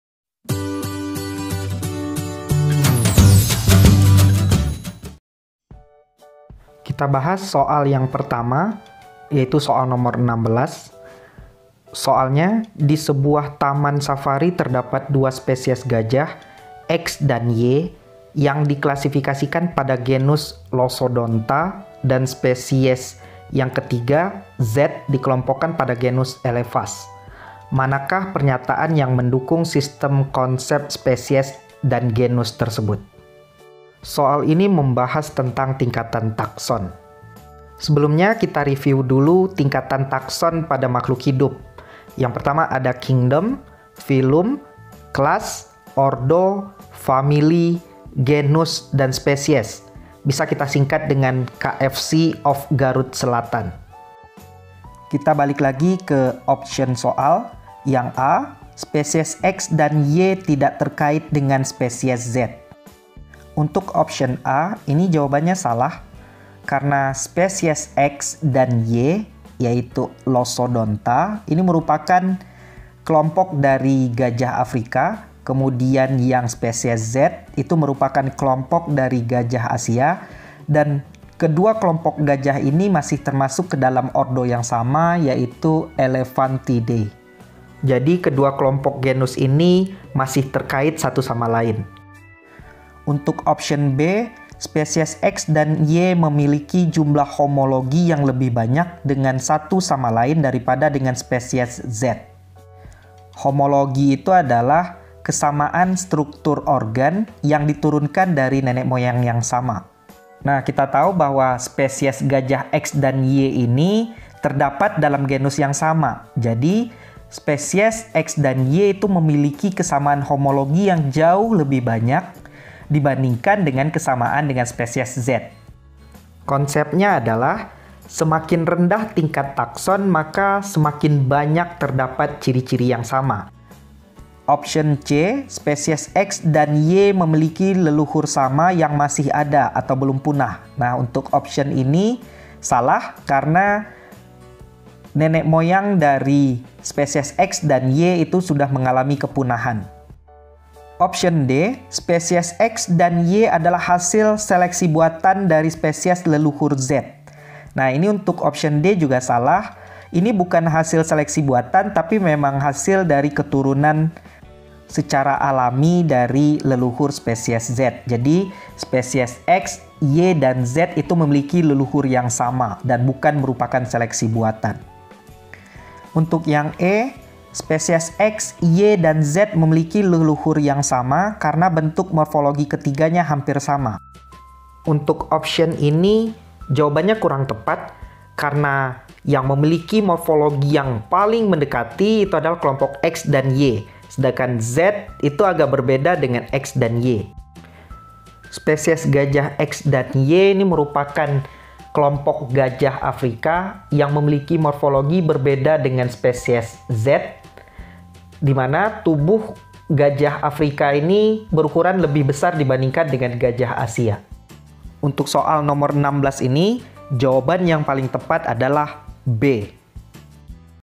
Pembahasan soal biologi SIMAK UI
Video ini berisi tentang pembahsan soal biologi simak ui 2019. Adapun soal ini khusus Soal simak ui 2019 atau Soal simak ui saintek, dan materi pelajaran Soal simak ui biologi.
Semoga pembahasan soal simak ui ini bermanfaat dan selamat menyaksikan.